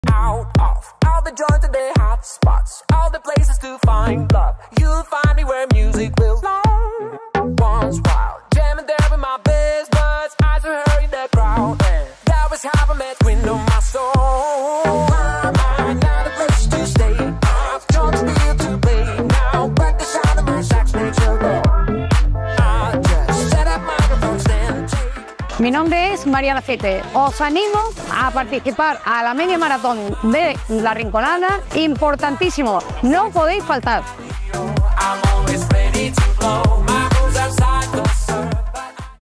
V Media Maraton La Rinconada Entreparques
Spot anunciador de la V Media Maraton La Rinconada Entreparques. Se celebrará el domingo 26 noviembre 2023 a las 10:00 h.

Puedes inscribirte através de la página Web del Patronato Municipal de Deportes de La Rinconada

Salida y meta desde el Parque de Las Graveras, pudiendo participar toda aquella persona que tenga cumplidos 18 años en dicha fecha.